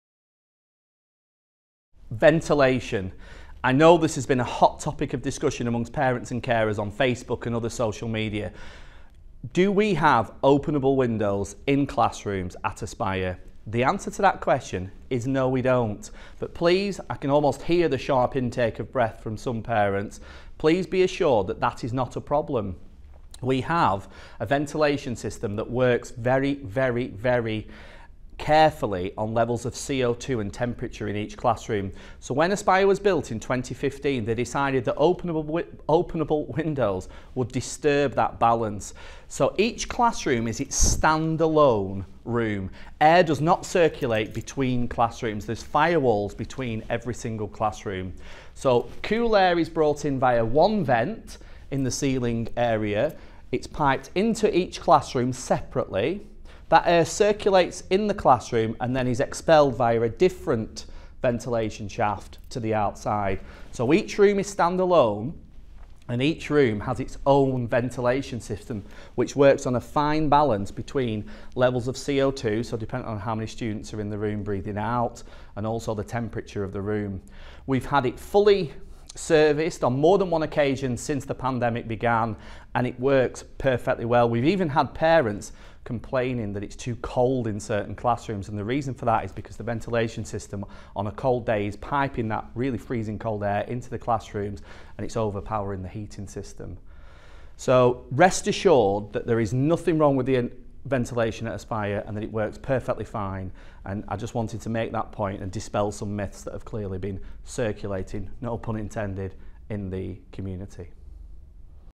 Ventilation at Aspire
In this short video Mr Woods explains the ventilation system at Aspire.